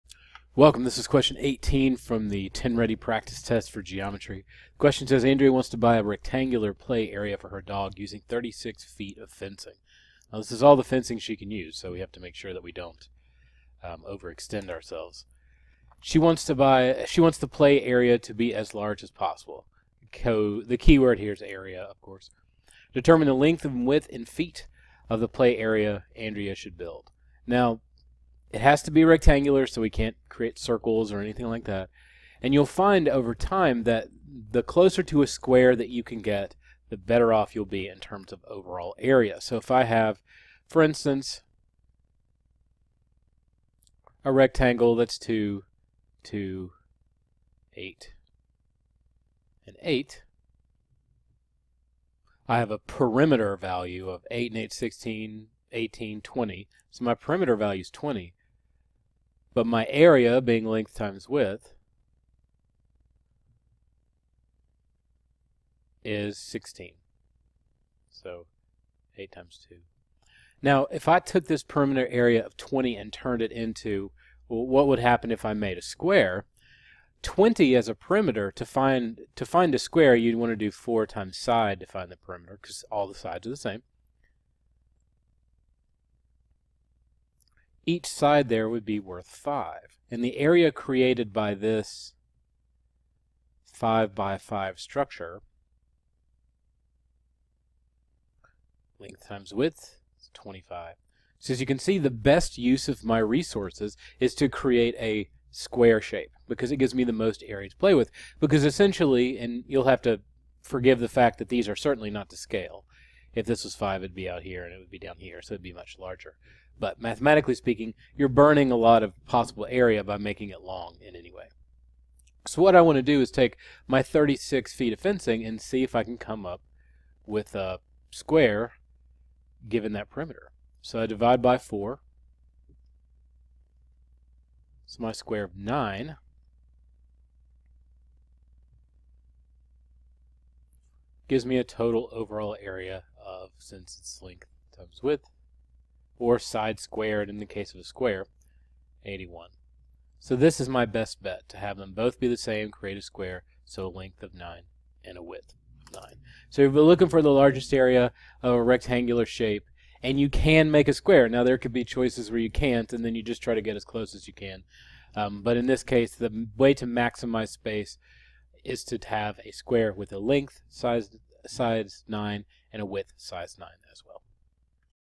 Question 14 (Used to be 18) - Geometry - TNReady Practice Test
**In this older version, they asked for both the length and the width, but in the new version they only want the width.  Make sure that you provide them the value for which they asking. They are trying to trick you, but you don't have to fall for it.**